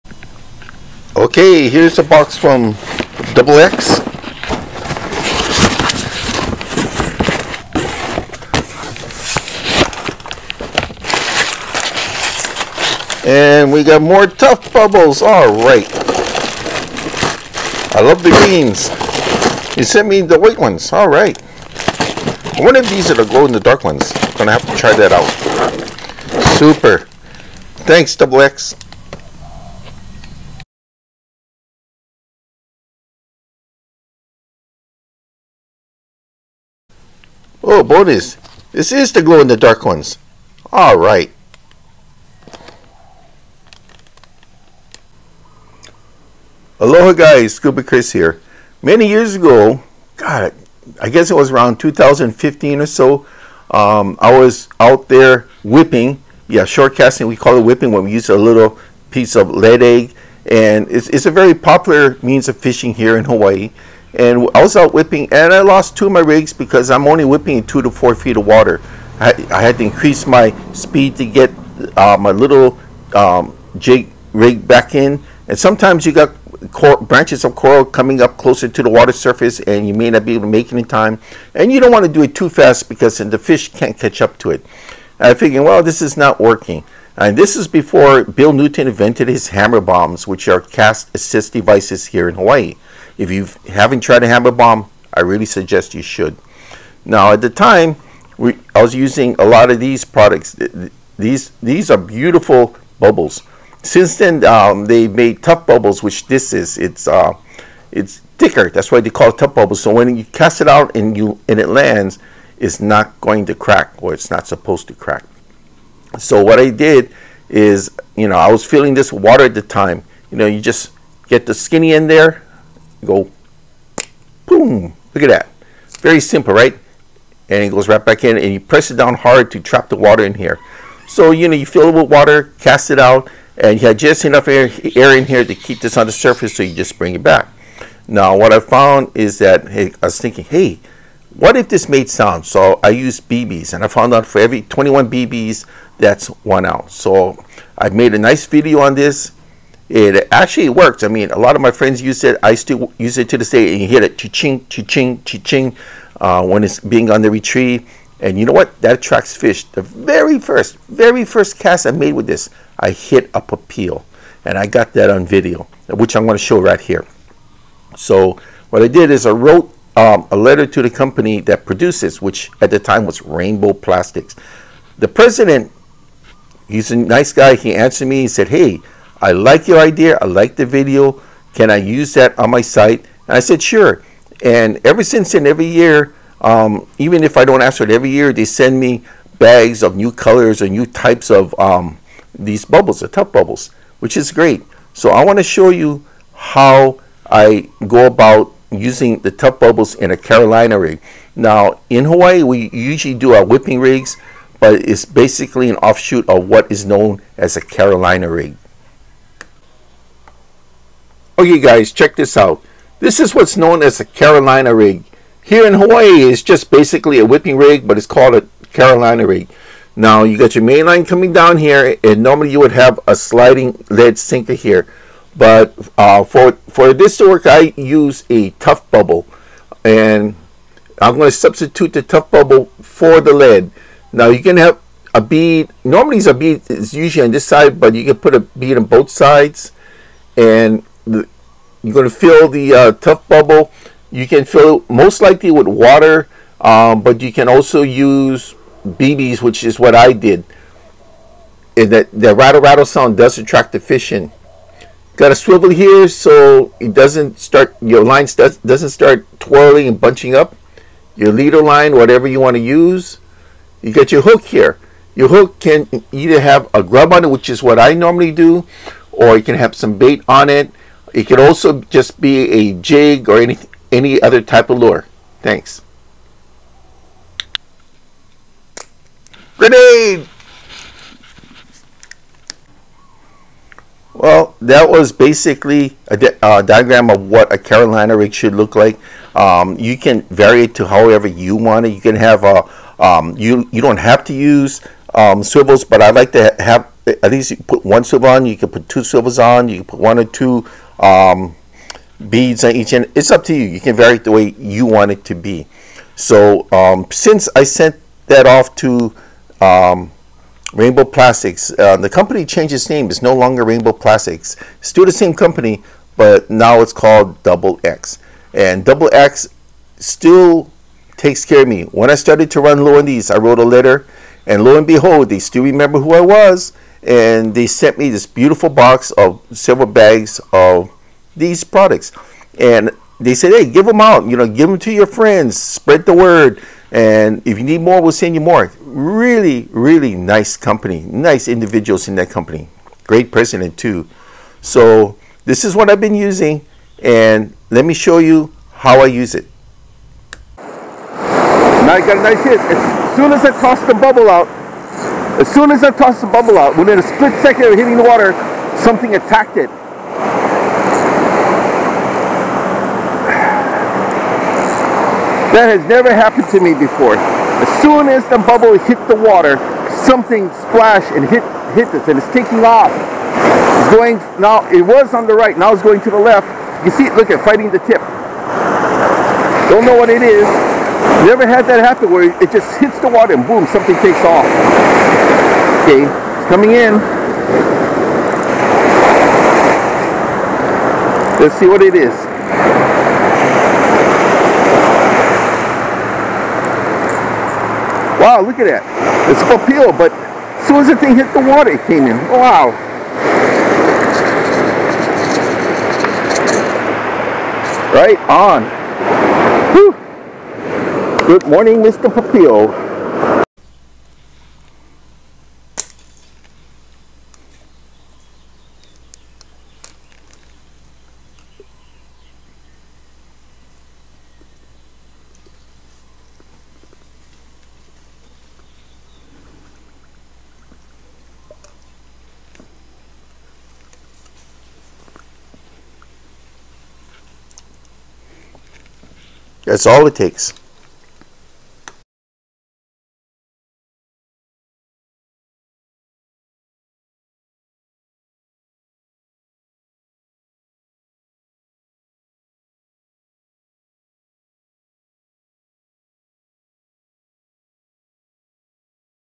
Carolina Rig Using Tough Bubble Float by Double X!
Rainbow Plastics is Now Double X!

Years ago i modified a Rainbow Plastic Tough Bubble by using BB's instead of water. I theorized that the noise of the BB's would attract predators. I was right. On my very 1st cast i caught a Papi'o "Trevally Jack". I emailed Rainbow Plastics about what i did & the President of the company asked me if they could incorporate my modification into their line. No problem. Since then the NEW Rainbow Plastics which is now Double X will supply whatever i need. Thank you Double X!

Cast Bubble 2" Med T.Green 3Pk Fishing Products

HiFishGear Hawaii. Fastest shipping company I know off! Shipped in 24hr of receipt of order!